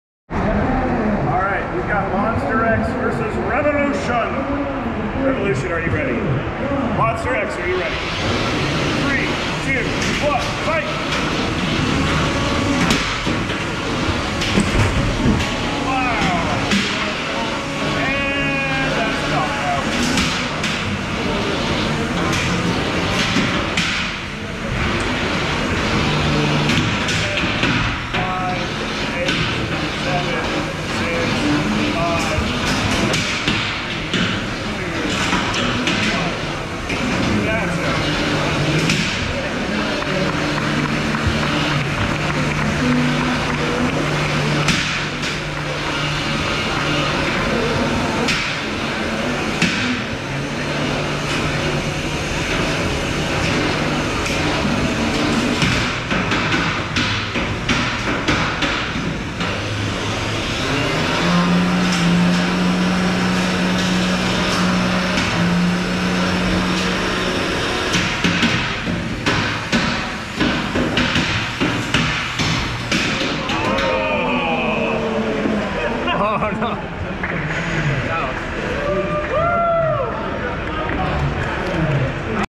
Scalar vs Hyperion Loser's Bracket Match 3 Antweight Combat Robogames 2024
I was running Scalar for Peter Garnache.